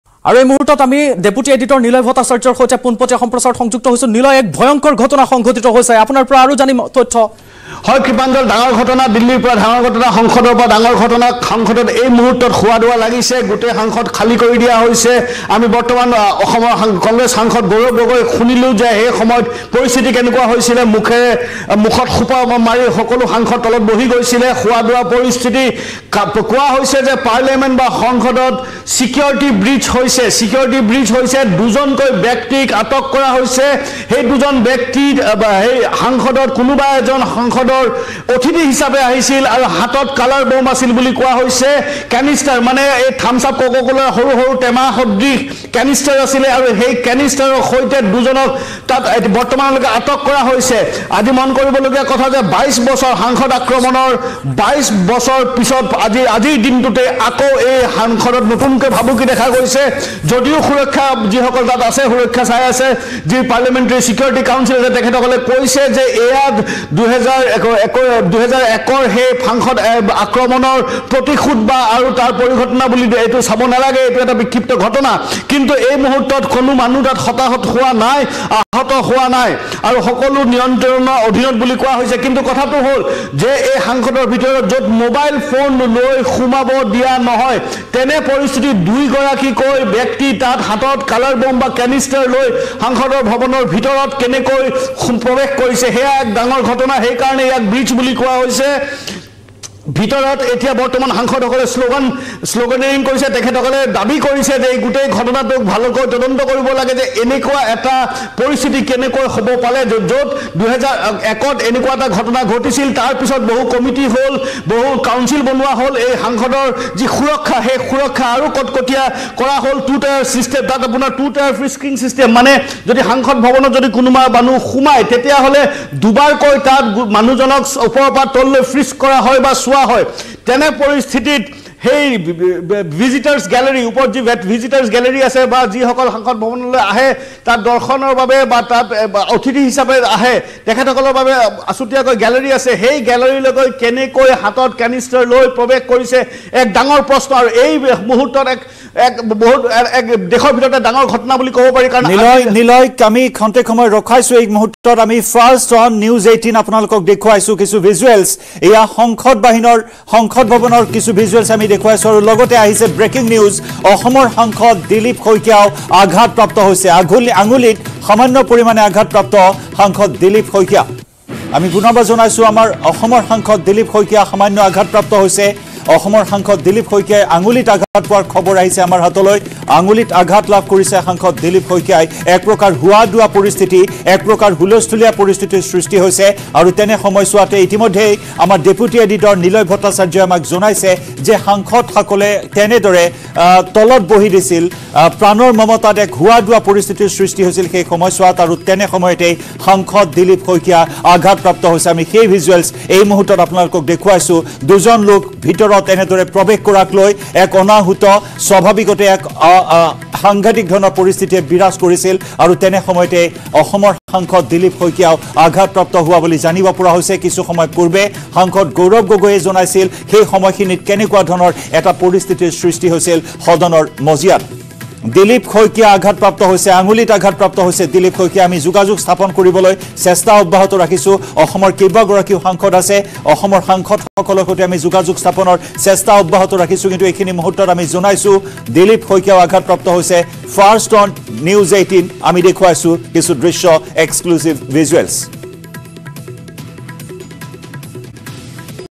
Parliament Attack | সংসদ ভৱন আক্ৰমণৰ বৰ্ষপূৰ্তিৰ দিনটোতে নিৰাপত্তাত বৃহৎ খুঁট
Parliament Attack |  লোকসভাৰ মজিয়াত দুৰ্বৃত্তৰ আতংক।
সংসদ ভৱন আক্ৰমণৰ বৰ্ষপূৰ্তিৰ দিনটোতে নিৰাপত্তাত বৃহৎ খুঁট।
লোকসভাৰ অধিৱেশন চলি থকাৰ সময়তে ভিজিটৰছ গেলাৰীৰ পৰা হঠাৎ জঁপিয়ালে দুজন লোকে।
CCTV কেমেৰাত বন্দী হ’ব জঁপিওৱাৰ দৃশ্য। জঁপিয়াই লোকসভাৰ মজিয়ালৈ দৌৰি যায় লোক দুজনে। লগে লগে তৎপৰতাৰে সদনৰ ভিতৰত জঁপিওৱা দুৰ্বৃত্তক আটক কৰে নিৰাপত্তাৰক্ষীয়ে। আটক কৰাৰ পিছতে শ্লোগান দিয়ে দুৰ্বৃত্তই। জঁপিওৱাৰ সময়তে একাংশ সাংসদে ধৰিবলৈ চেষ্টা কৰিছিল দুৰ্বৃত্তক। এই ঘটনাত অসমৰ সাংসদ দিলীপ শইকীয়া সামান্য ভাৱে আঘাতপ্ৰাপ্ত হয়।